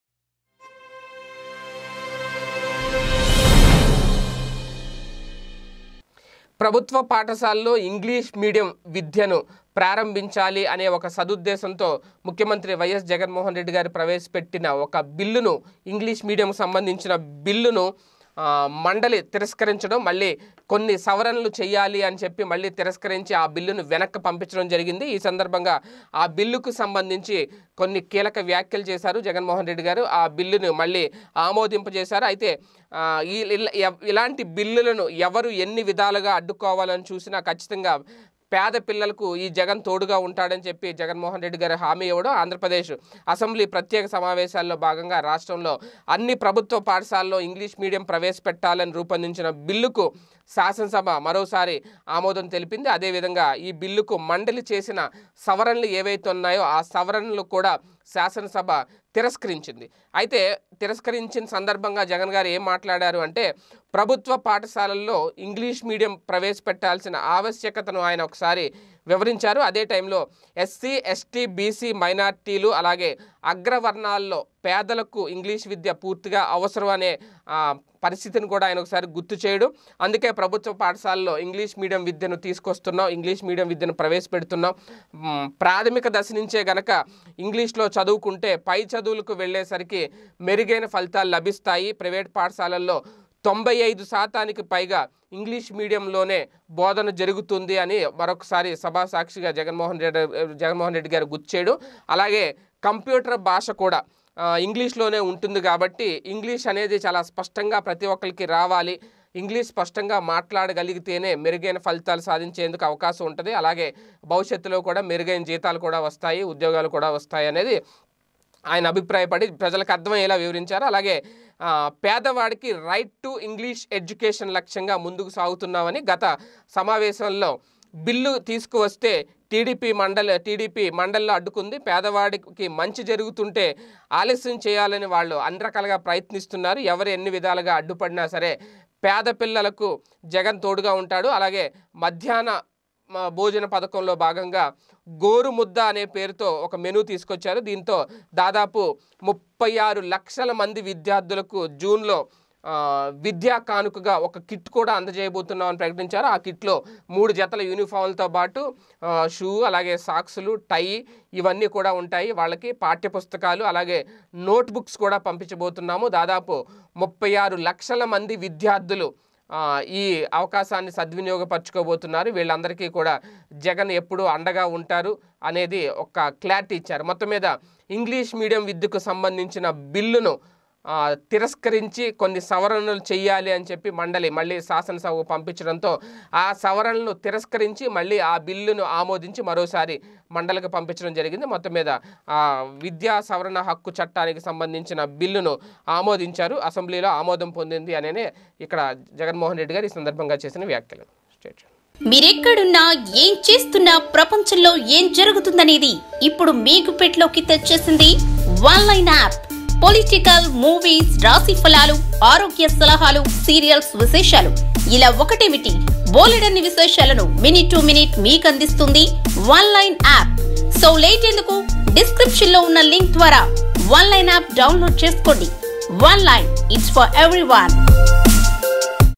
AP CM Jagan On English Medium | 36 లక్షల మంది విద్యార్ధులకోసం జగన్ అద్భుత నిర్ణయం
36 లక్షల మంది విద్యార్ధులకోసం జగన్ అద్భుత నిర్ణయం | AP CM Jagan On English Medium | PDTV News